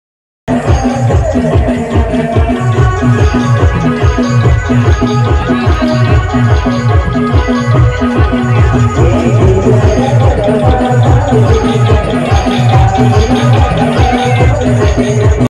विकास बैंड का सुपरहिट धमाका दिवाली पर सिलदड़ में मौज कर दिया दोस्तों 2022
आदिवासियों का सुपरहिट धमाका दिवाली की स्पेशल वीडियो